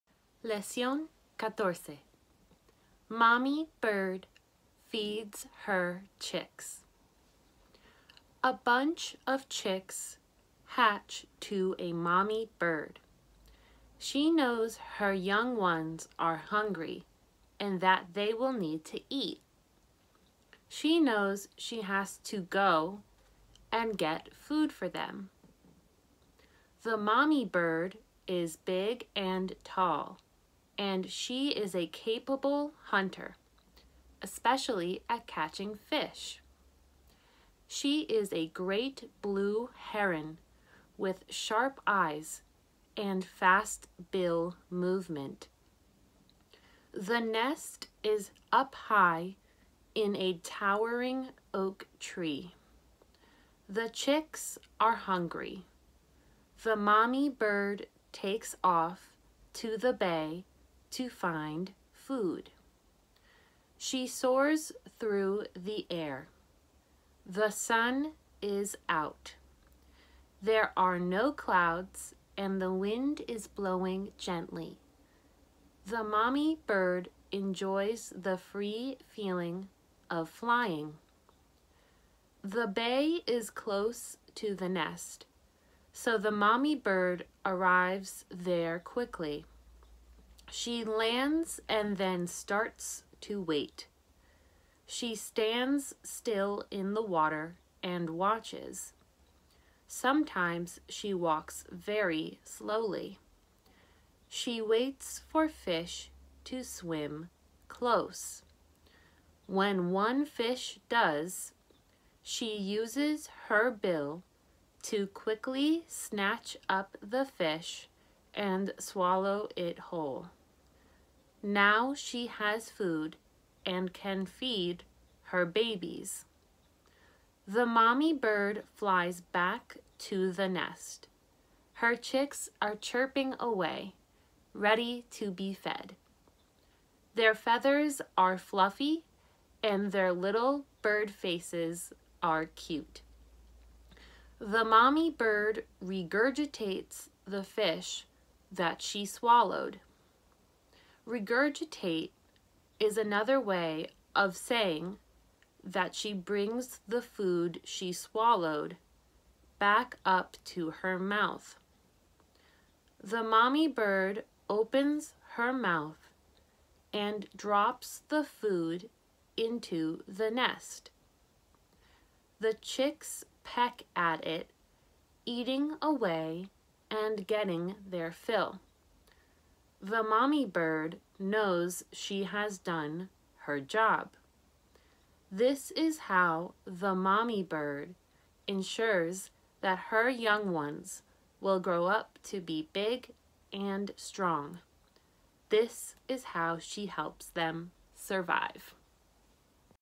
Leccíon número 14: “Mommy Bird Feeds Her Chicks”
Mommy Bird Feeds Her Chicks

A bunch of chicks hatch to a mommy bird. She knows her young ones are hungry and that they will need to eat. She knows she has to go and get food for them.

The mommy bird is big and tall, and she is a capable hunter, especially at catching fish. She is a great blue heron with sharp eyes and fast bill movement.

The nest is up high in a towering oak tree. The chicks are hungry. The mommy bird takes off to the bay to find food.

She soars through the air. The sun is out. There are no clouds, and the wind is blowing gently. The mommy bird enjoys the free feeling of flying.

The bay is close to the nest, so the mommy bird arrives there quickly. She lands and then starts to wait. She stands still in the water and watches. Sometimes she walks very slowly. She waits for fish to swim close. When one fish does, she uses her bill to quickly snatch up the fish and swallow it whole. Now she has food and can feed her babies.

The mommy bird flies back to the nest. Her chicks are chirping away, ready to be fed. Their feathers are fluffy, and their little bird faces are cute. The mommy bird regurgitates the fish that she swallowed. Regurgitate is another way of saying that she brings the food she swallowed back up to her mouth.

The mommy bird opens her mouth and drops the food into the nest. The chicks peck at it, eating away and getting their fill. The mommy bird knows she has done her job. This is how the mommy bird ensures that her young ones will grow up to be big and strong. This is how she helps them survive.